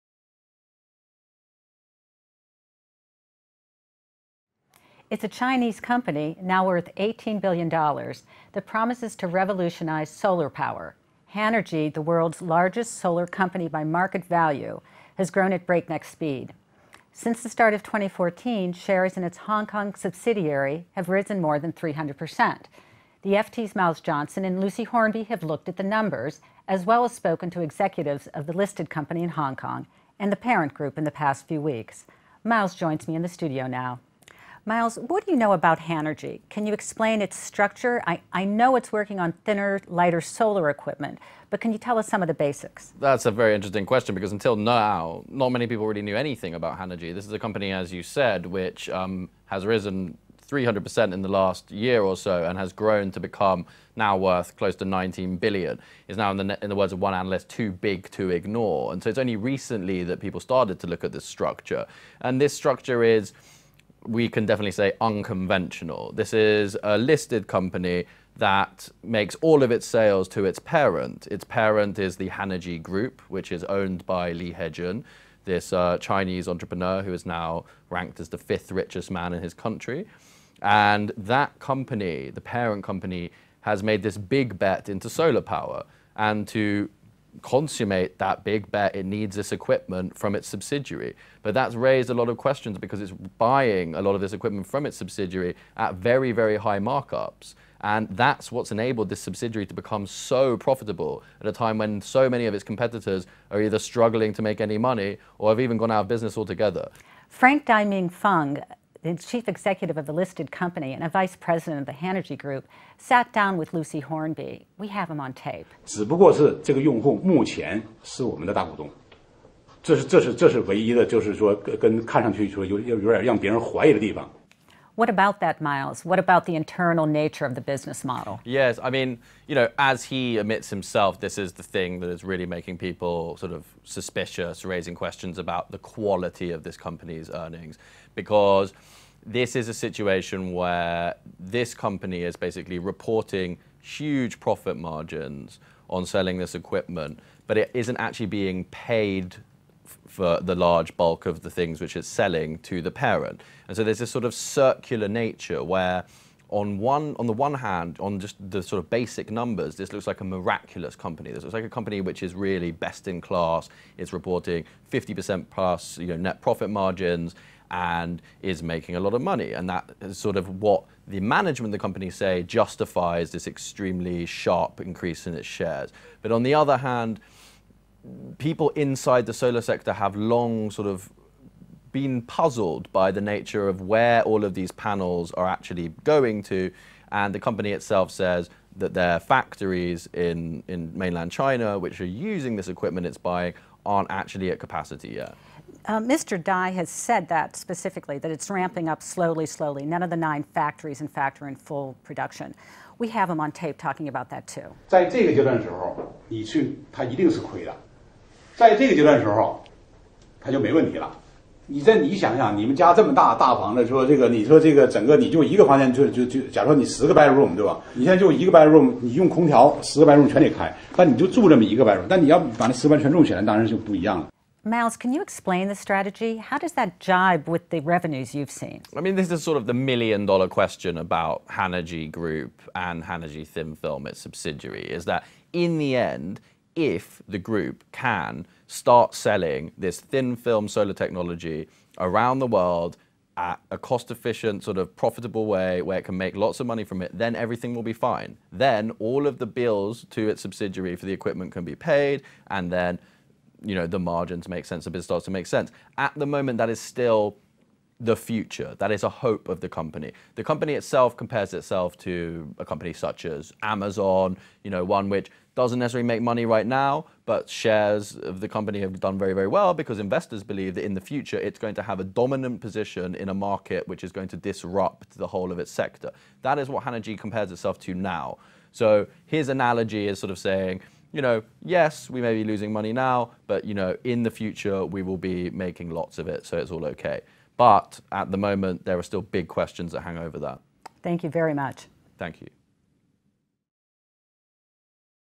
Hanergy's unusual business | FT Business
The FT has revealed some unconventional practices within Hanergy Group, the $18bn solar power Chinese company. Miles Johnson, hedge fund correspondent, explains the findings to investigations editor Christine Spolar.